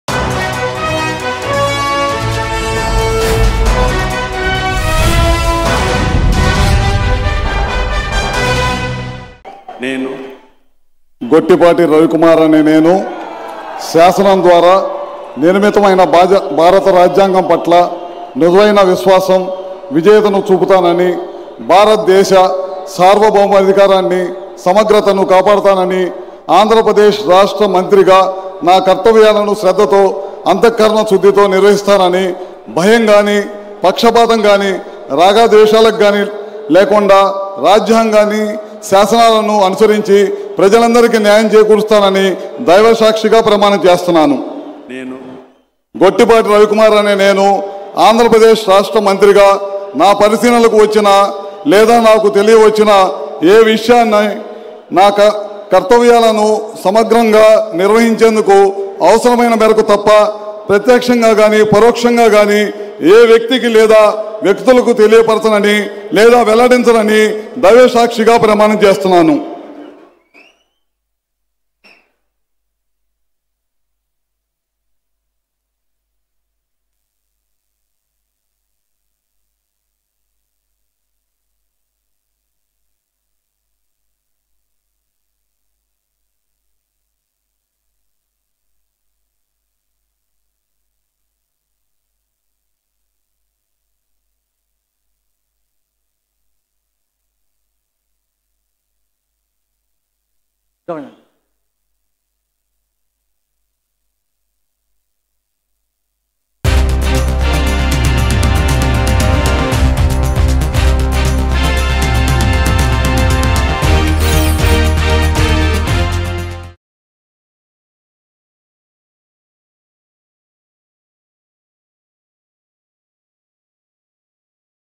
Gottipati Ravi Kumar Takes Oath As AP Cabinet Minister | AP Cabinet Ministers 2024 Oath Ceremony
Gottipati Ravi Kumar Takes Oath As AP Cabinet Minister. Nara Chandrababu Naidu took oath as the Chief Minister of Andhra Pradesh at Kesarapalli IT Park, Gannavaram, Krishna District. The ceremony was administered by AP Governor Syed Abdul Nazeer. Chandrababu, along with other members of the AP Cabinet Ministers, also took the oath.